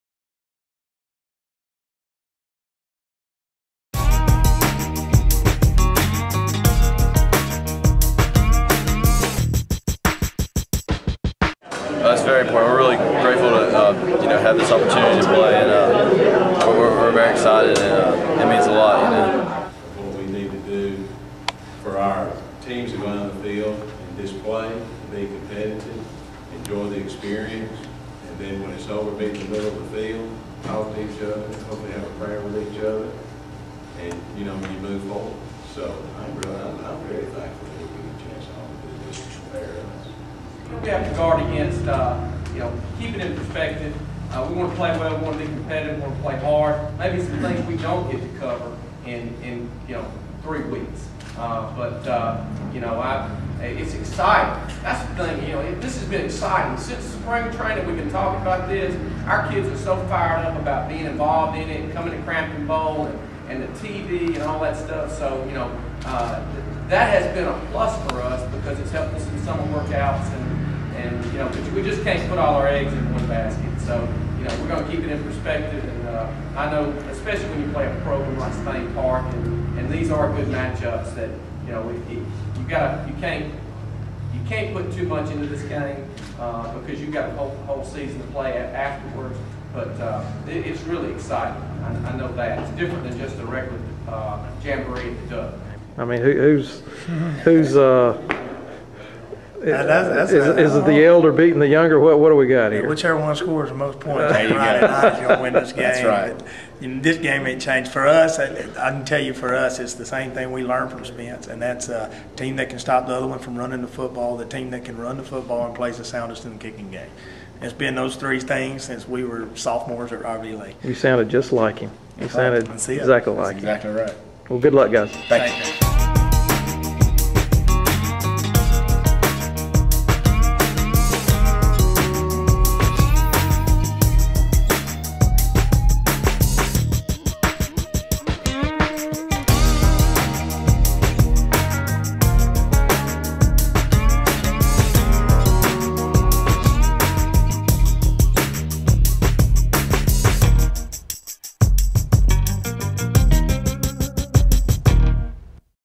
Champions Challenge Press Conference
Coaches and players from the 2010 Champions Challenge gathered at the offices of the Alabama High School Athletic Association for a press conference on Aug. 11, 2010. The participants include Trinity, Jackson, Opelika, Greenville, Spain Park and Auburn. Champions Challenge is scheduled the weekend of Aug. 27-28 at Cramton Bowl in Montgomery, Ala.